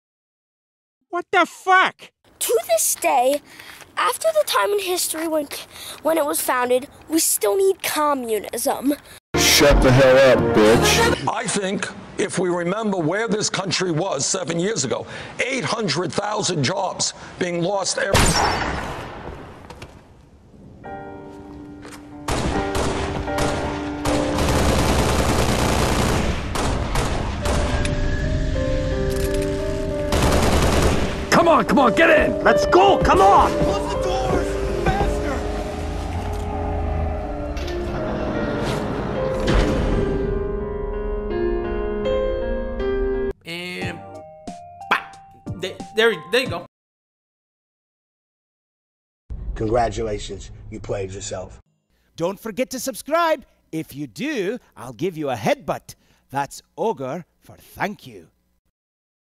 The Fall of Bernie
An accurate description of how barnie flip-flops got gamershot by the DNC, which allowed drumpf to win.